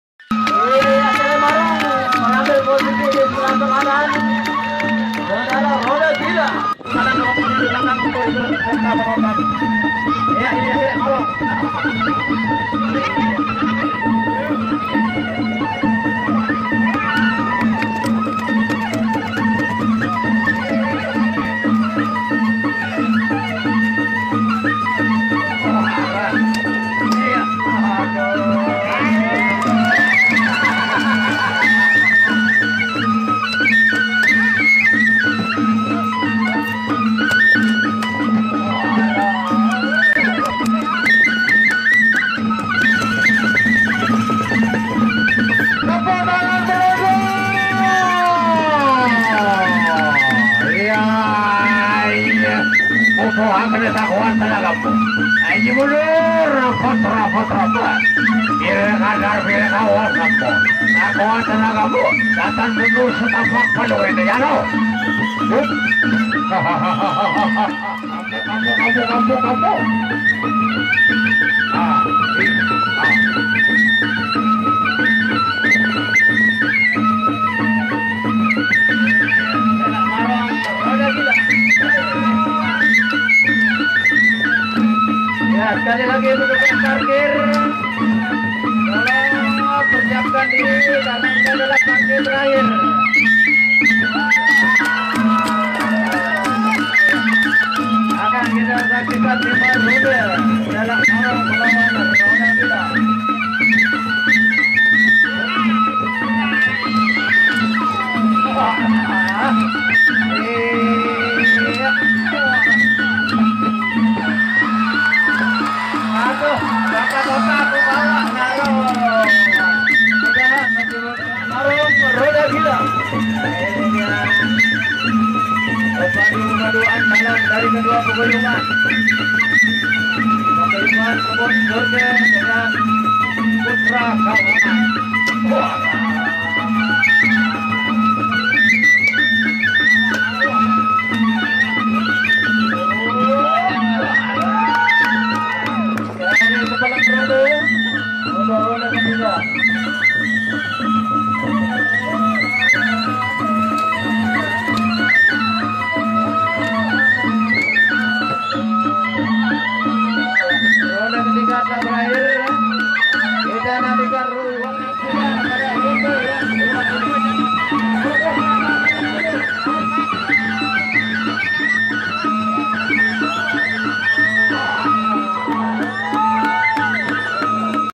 saling serang Adu kekuatan masing masing
adat budaya seni yang berasal dari Lombok yang di juluki presean